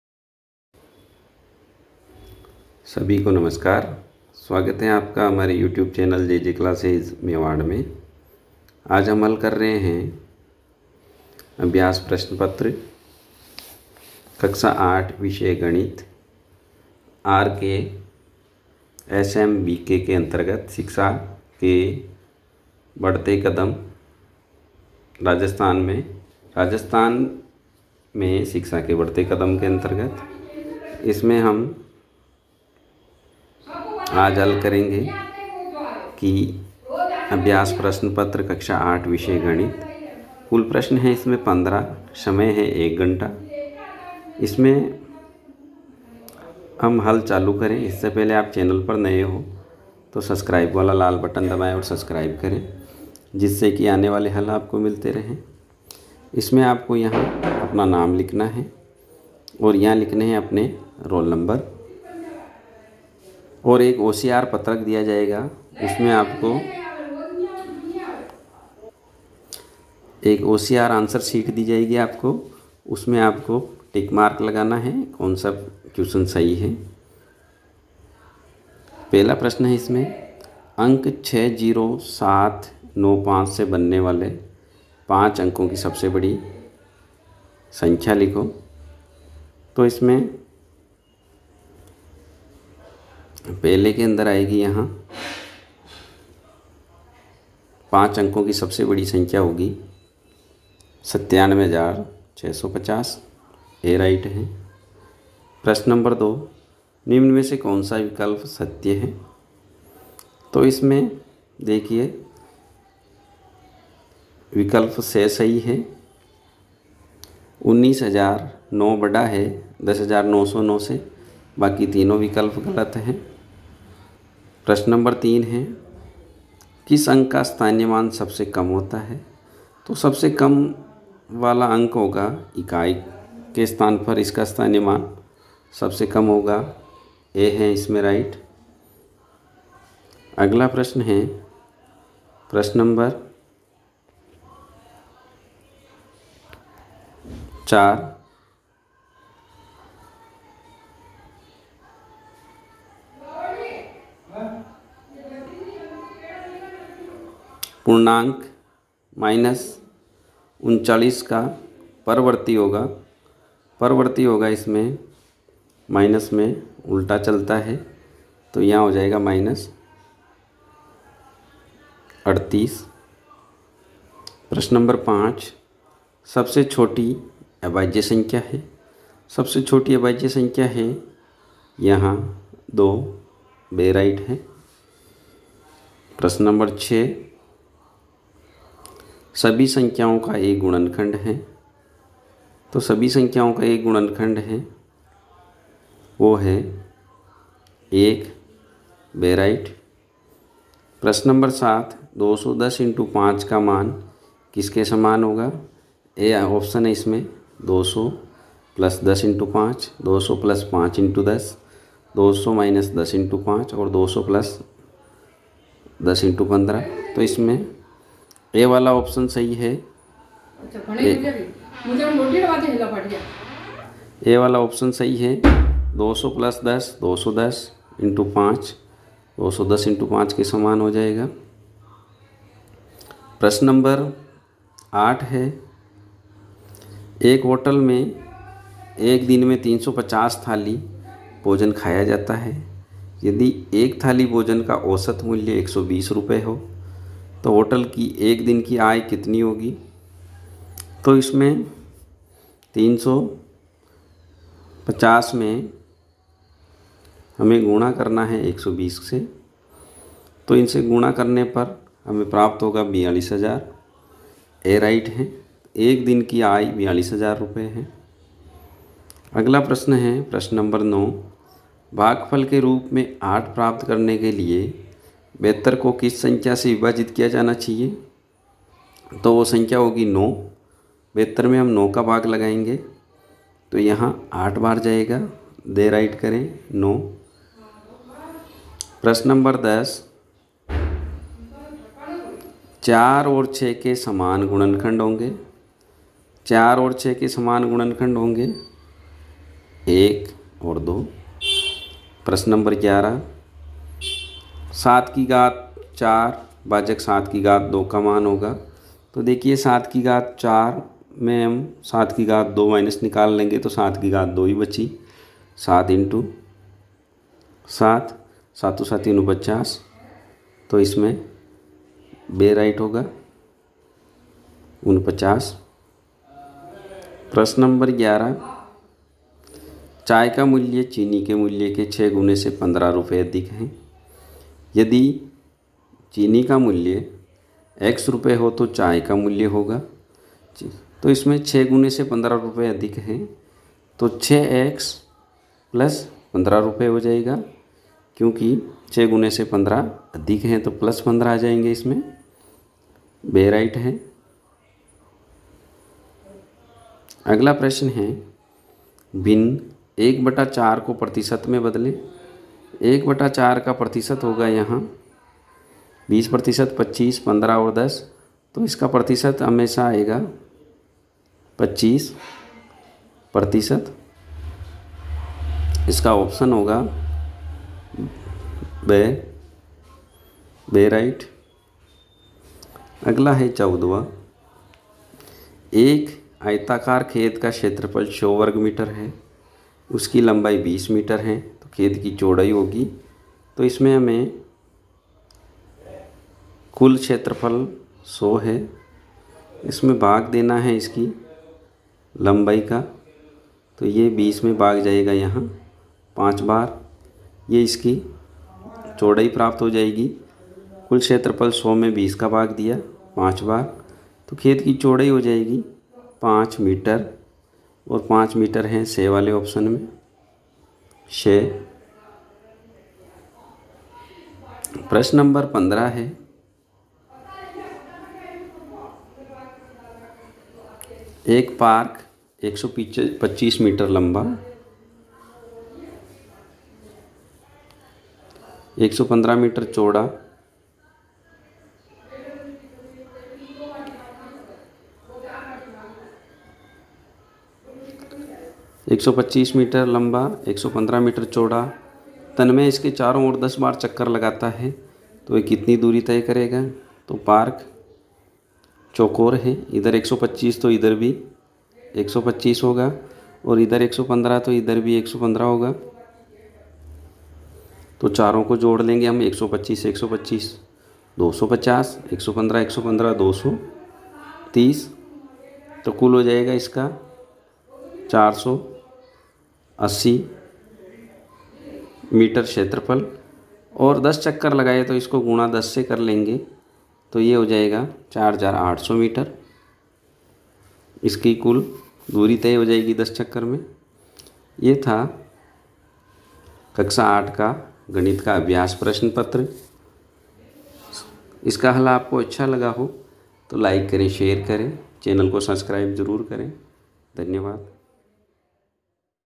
कक्षा 8 गणित अभ्यास प्रश्न पत्र | RKSMBK | Class 8 maths paper | राजस्थान में शिक्षा के बढ़ते कदम
राजस्थान के शिक्षा में बढ़ते कदम पर आधारित अभ्यास प्रश्न पत्र 11 अक्टूबर से 18 अक्टूबर के मध्य लिया जाएगा
इस पेपर का हल हम आपको उपलब्ध करवा रहे हैं

2 से 4 नवंबर परीक्षा
दो से चार नवंबर परीक्षा
2 to 4 navamber exam
3 Novemberकक्षा 8 गणित पेपर
क्लास 8 मैथ्स पेपर 3 नवंबर